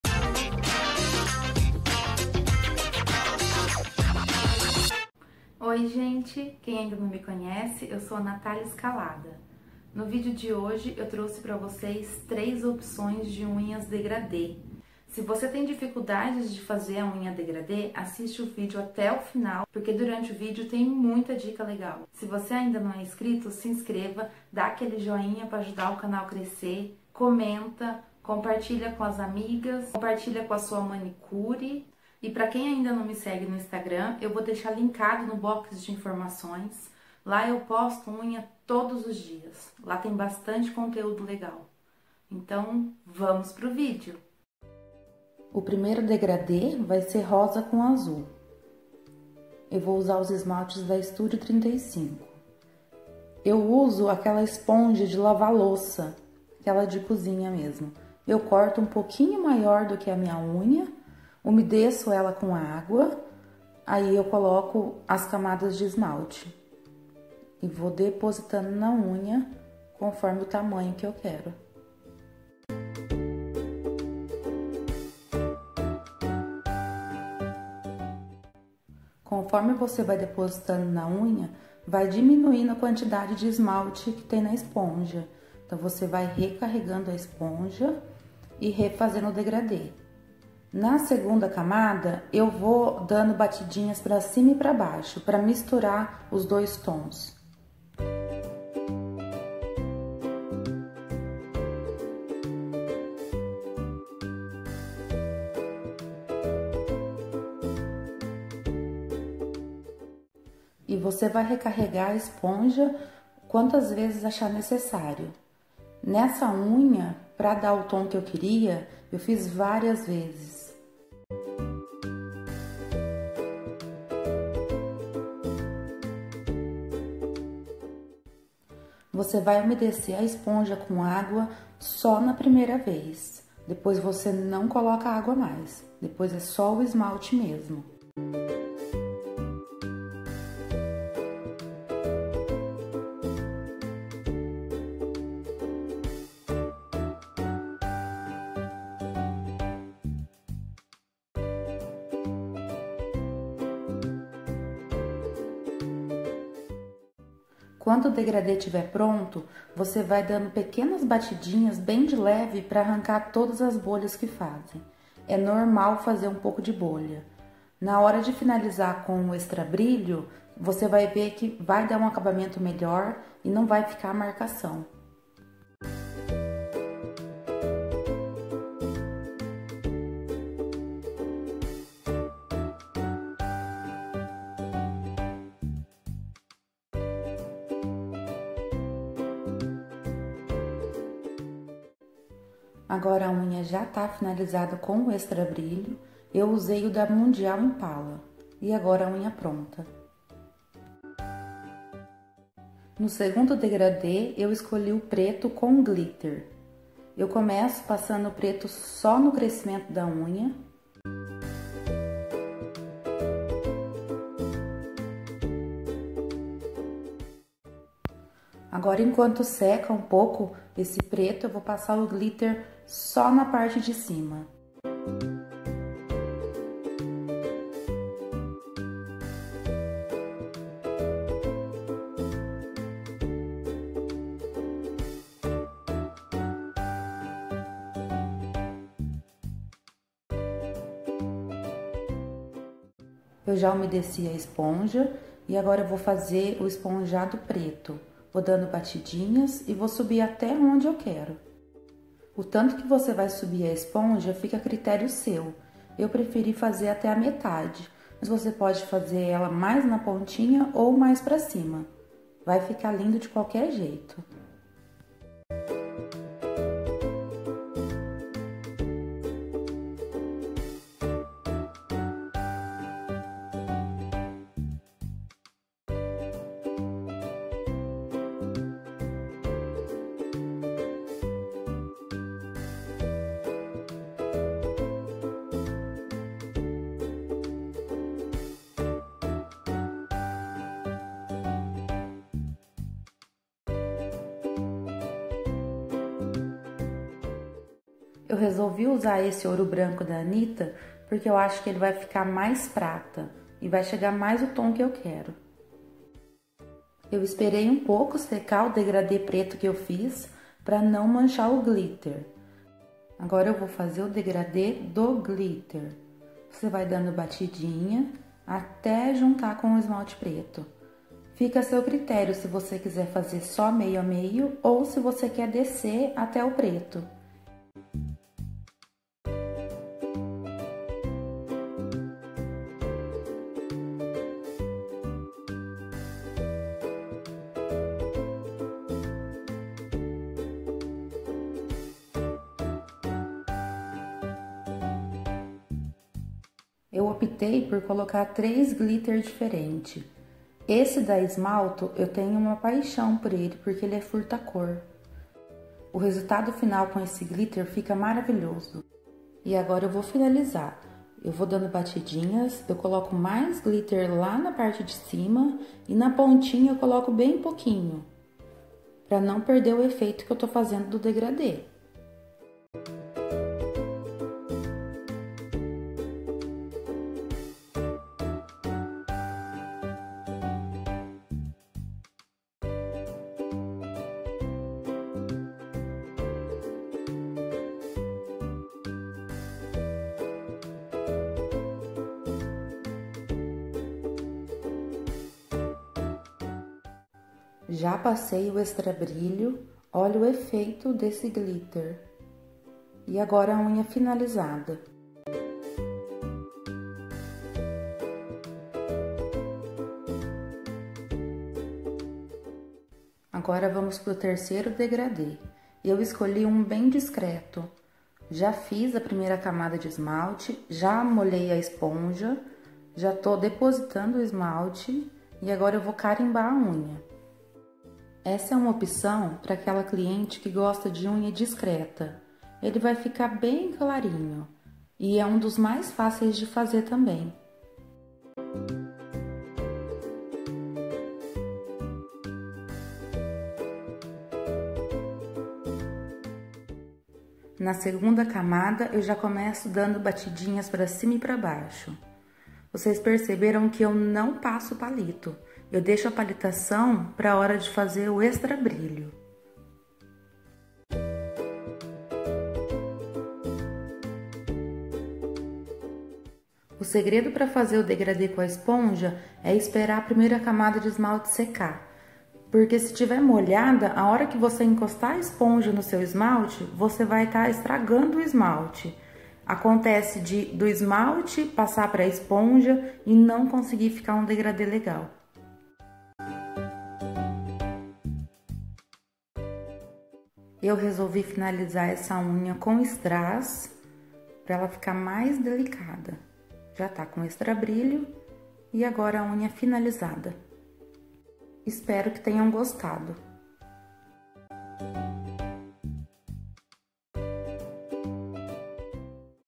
IDÉIAS DE UNHAS DEGRADÊ | 3 OPÇÕES
Nesse vídeo eu fiz o passo a passo de 3 opções de unhas degradê.  Mas vocês podem brincar com a cores, e fazer no tom que gostar mais.
Usei os esmaltes:
1°
Dear Rose e Jeans Rasgado ambos da marca Studio 35

2°
Preto da marca Mundial Impala
Glitter Diamante da marca Mohda
Glitter Ouro Branco da marca Anita
Glitter Prata 3D da marca Smalto

3°
Lágrima de unicórnio da marca Dailus
Chic Pele da marca Colorama

Assista também 👇

Você sabe a diferença de esmalte Free e esmalte Hipoalergênico?